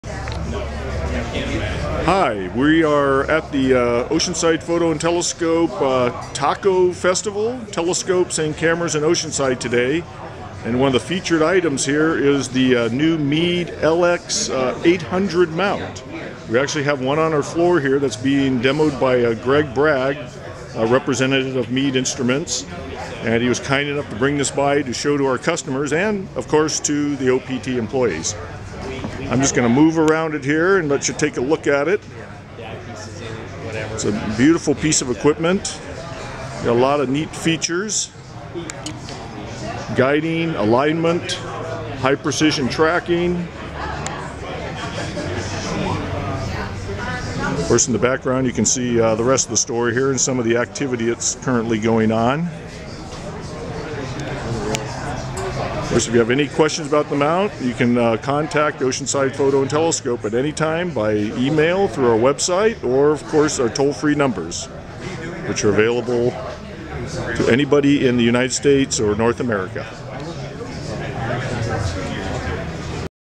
Meade LX-800 mount at OPT T.A.C.O. event
Meade makes a showing of their new LX-800 mount at OPT's annual T.A.C.O. event.